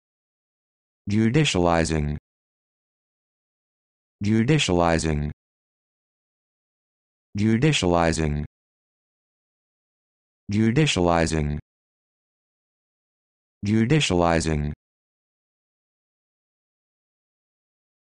How to Pronounce Judicializing
This video shows you how to pronounce Judicializing